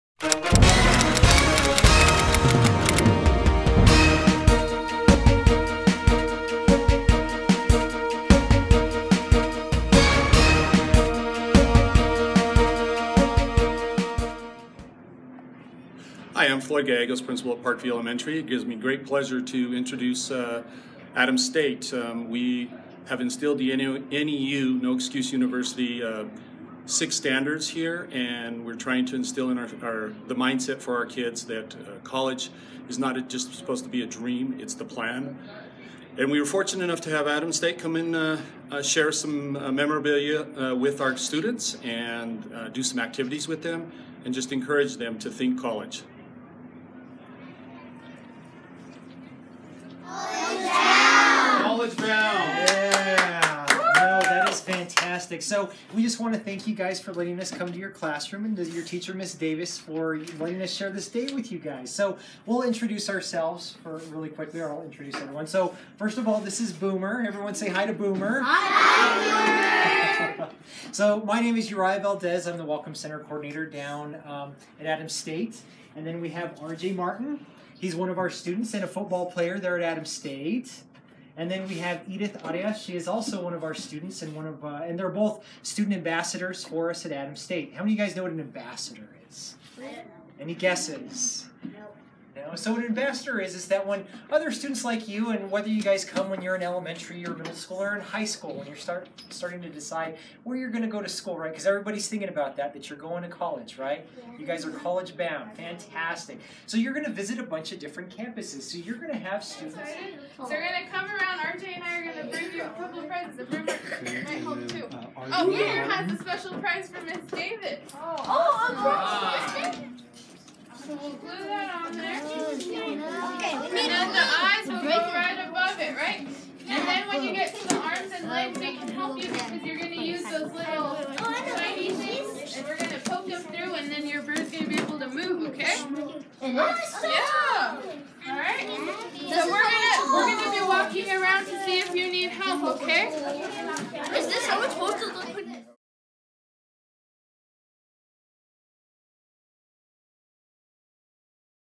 Park View Elementary Students College Day
Adams State University visits second graders as part of college readiness program